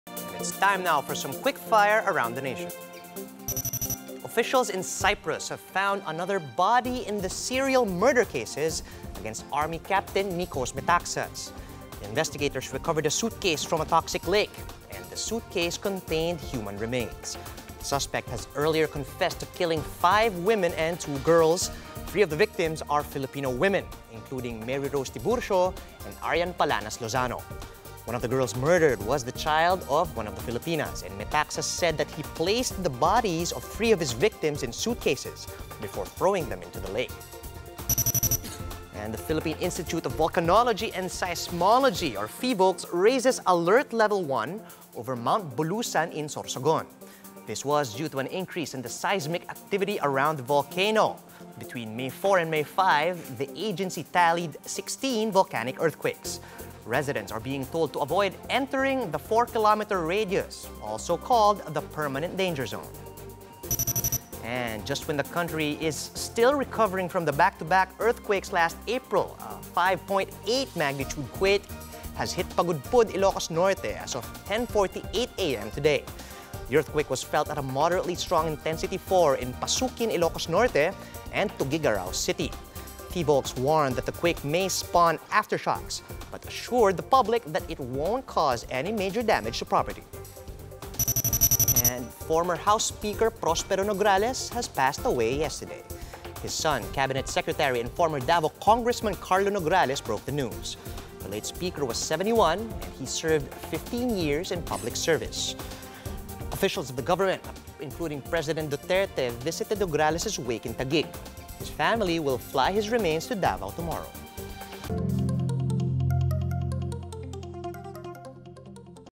OneNewsroom QUICKFIRE
• Cypriot officials recover suitcase containing human remains
• Phivolcs raises alert level 1 over Mt. Bulusan
• 5.8-magnitude quake hits Pagudpud, Ilocos Norte
• Tributes pour in for late House speaker Nograles